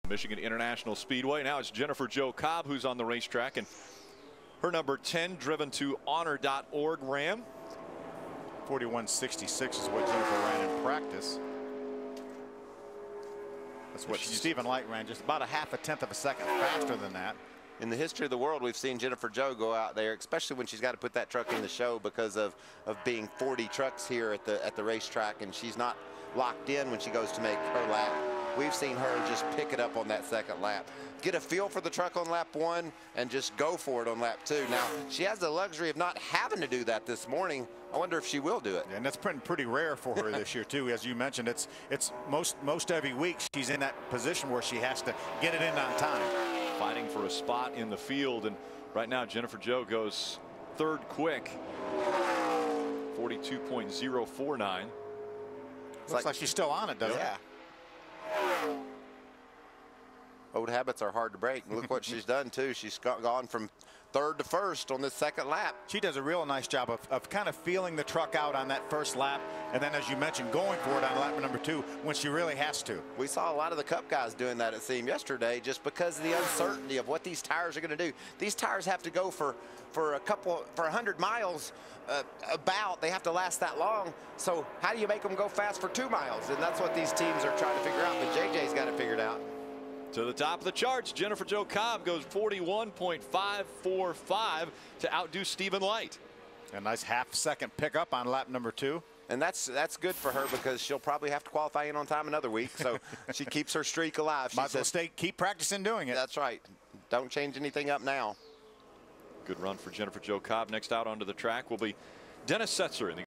Jennifer Jo Cobb - Michigan - Another "All Out" Qualifying Lap
Even when locked in the show, Jennifer Jo Cobb aims for the pole position. Video after video of using every HP to go "all out." This is what racing is all about. There is no quit in her and no quit in her fans! JJCR - Michigan International Speedway (8-18-12)